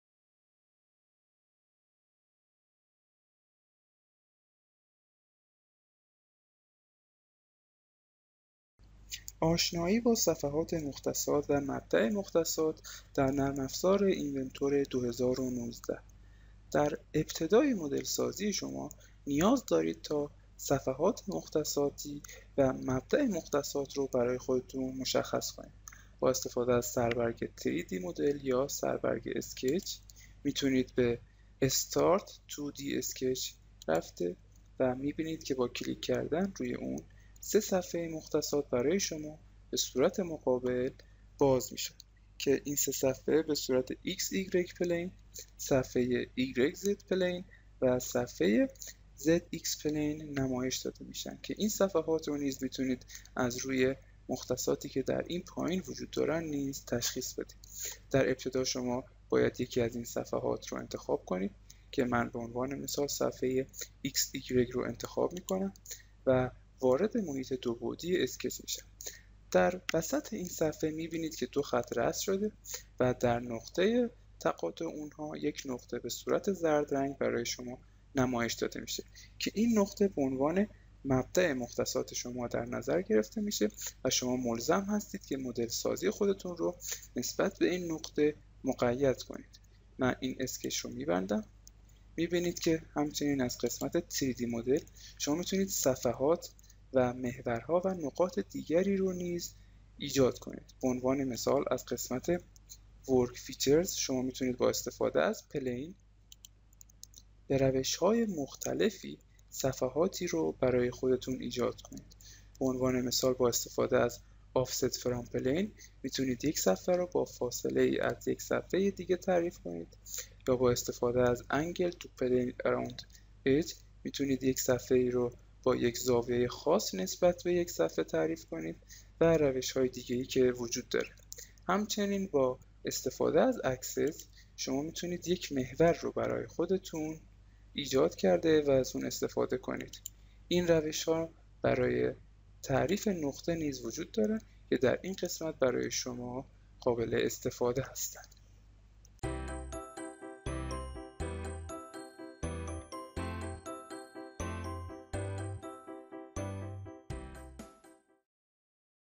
آشنایی با صفحات مختصاتplane و مبدا مختصات origin در نرم افزار اتودسک اینونتور 2019 (Autodesk In...
آشنایی با صفحات مختصاتplane  و مبدا مختصات origin در نرم افزار اتودسک اینونتور 2019 (Autodesk Inventor professional 2019) از دوره آموزش متوسطه نرم افزار اینونتور